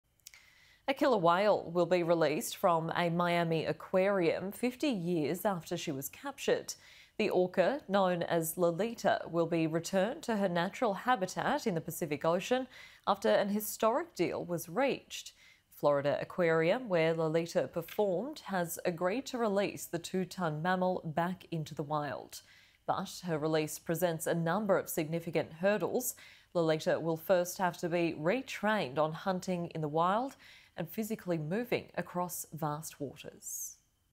Orca to be freed after 50 years in captivity
A killer whale will be released from a Miami aquarium 50 years after she was captured.

The orca, known as 'Lolita,' will be returned to her natural habitat in the Pacific Ocean after a historic deal was reached.

The Florida Aquarium, where Lolita performed, has agreed to release the 2-tonne mammal back into the wild.

However, her release presents several significant hurdles.

Lolita must first be retrained on hunting in the wild and physically moving across vast waters.